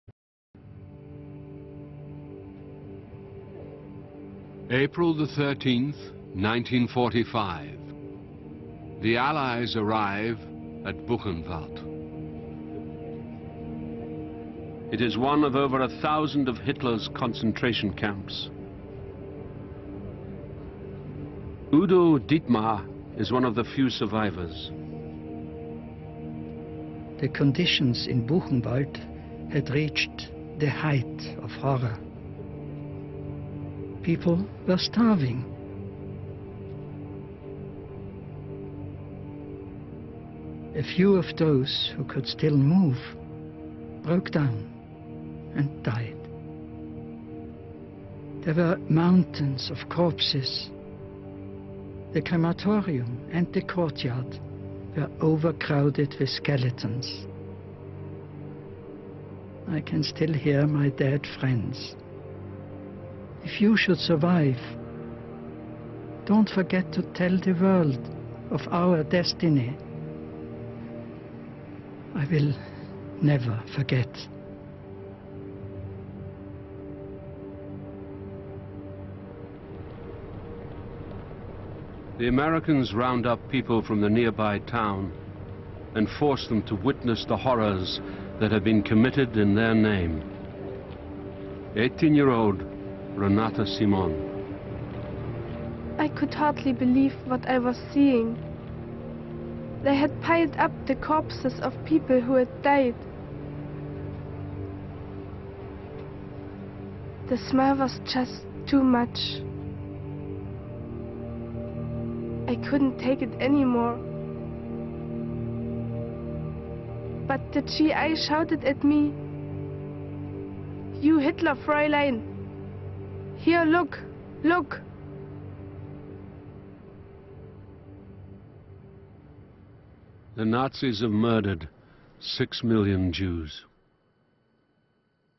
hitler in color (one of the concentration camps)
the allies reach one of the concentration camps